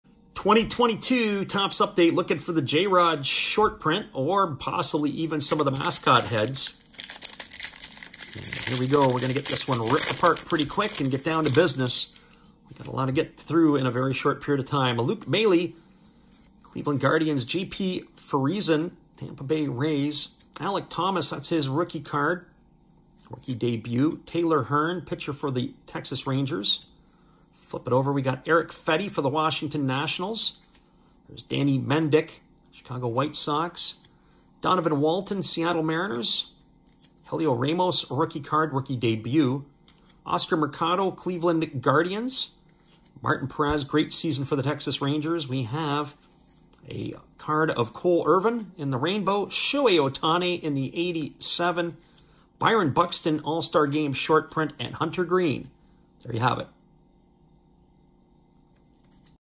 2022 Topps Update Baseball Shohei Ohtani Insert
2022 Topps Update Baseball is out and it is loaded!!  In this pack we snagged a Shohei Ohtani insert.  We are cracking packs from Blaster Boxes over the next couple of weeks, and we are taking you folks on the ride with us!!  The Mascot Big Head Short Prints are like finding diamonds!!  However, we are comfortable pulling any of the hot rookies!!    Lots of future stars in this episode.  Check out the product and let us know what you think!!